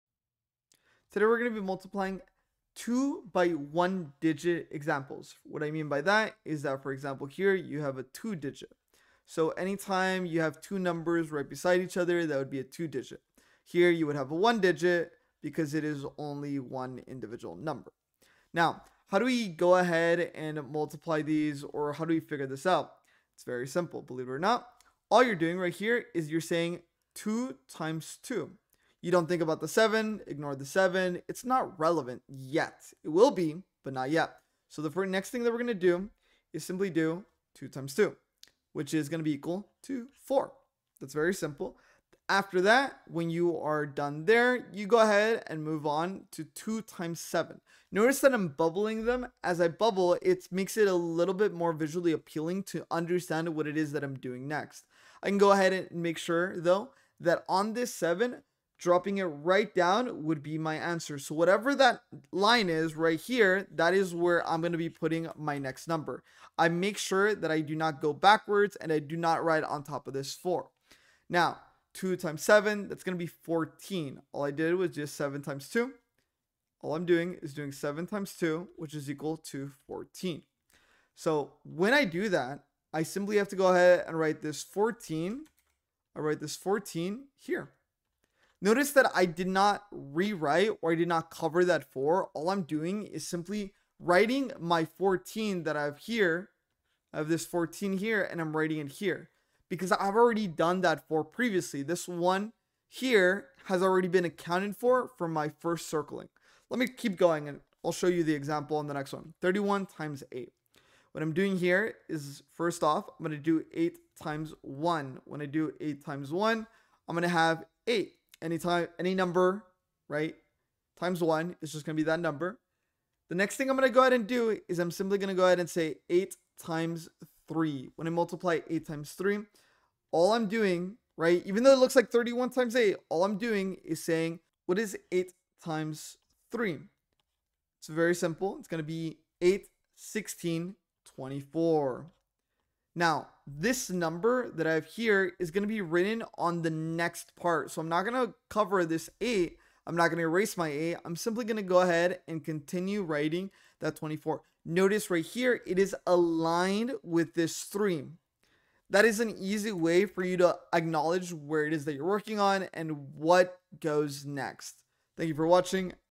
How To Multiply 2-Digit by 1-Digit | Elementary Math
Master the basics of multiplication with this easy-to-follow tutorial on multiplying 2-digit numbers by 1-digit numbers! 📚💡 Whether you're a student learning multiplication for the first time or a parent helping your child with math homework, this step-by-step guide will break it down in a simple and engaging way.

In this video, you'll learn:
✅ How to multiply using place value
✅ The step-by-step process for solving problems
✅ Helpful tips for carrying numbers correctly
✅ Real-world examples to reinforce learning

Perfect for elementary students, teachers, and parents looking for a clear and effective math tutorial.

📢 Share this with a friend who needs math help!